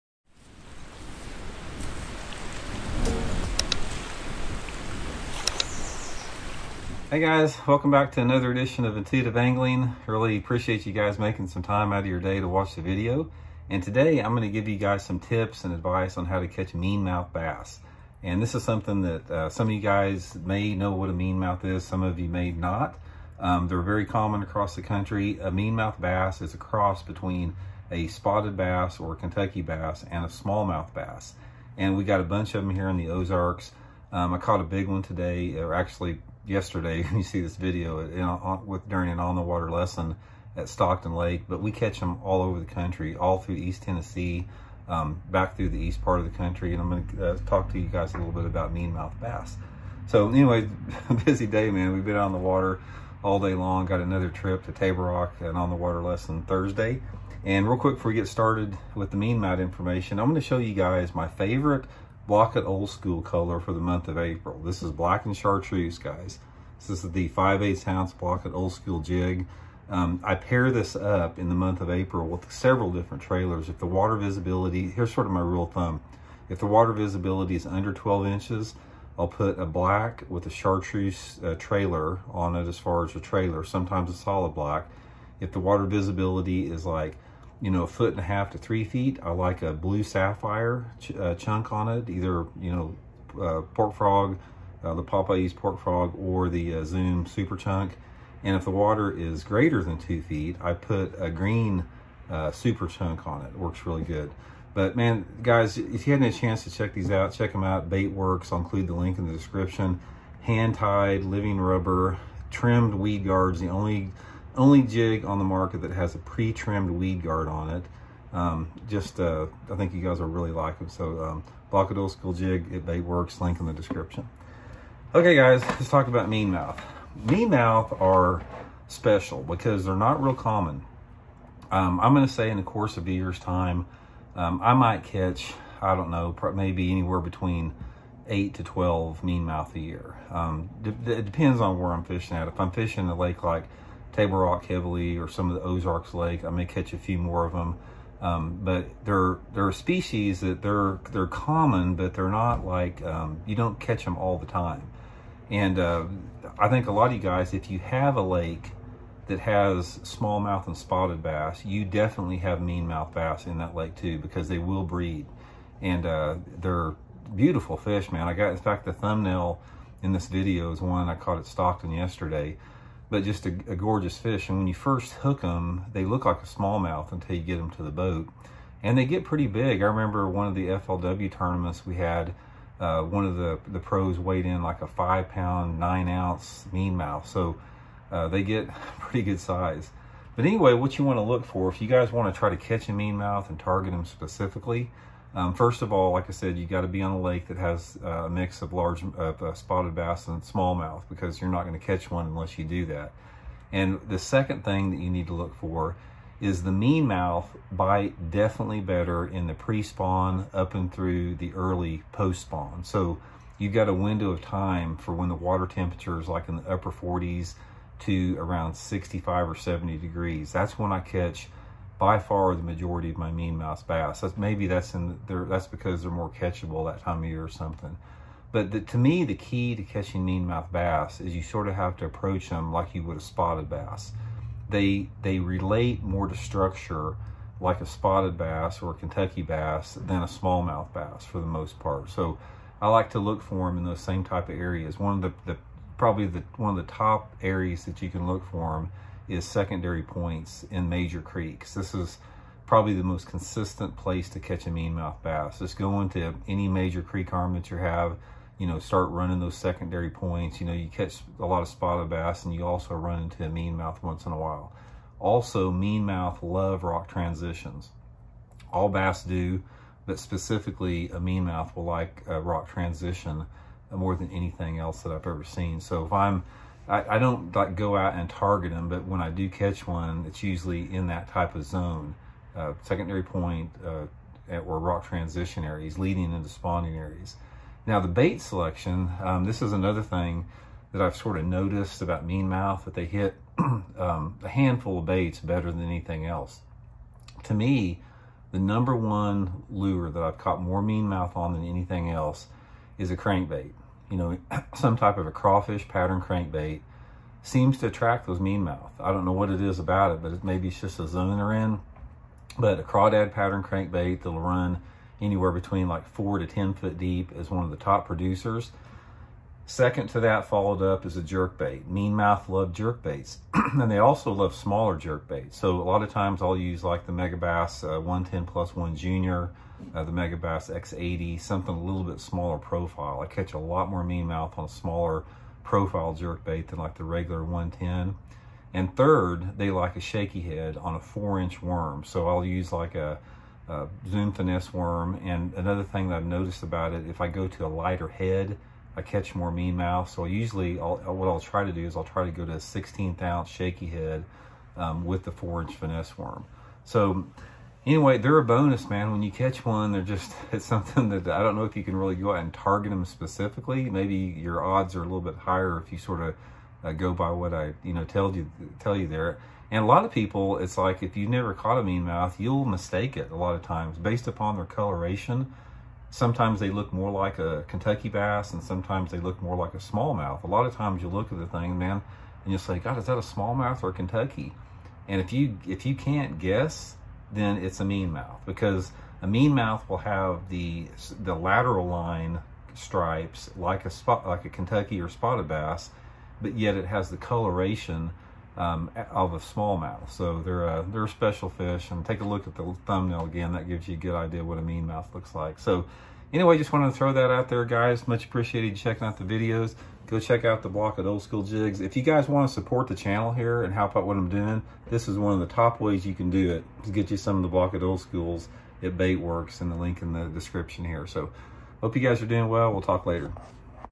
3 Secrets For Catching A Big MeanMouth Bass…
Randy give some tips and advice on how to catch mean mouth bass which is a cross between a smallmouth and a spotted bass…bassfishing fishthemoment fishing bass smallmouthbass spottedbass bassmaster toyotaseries bassmasteropens mlf ￼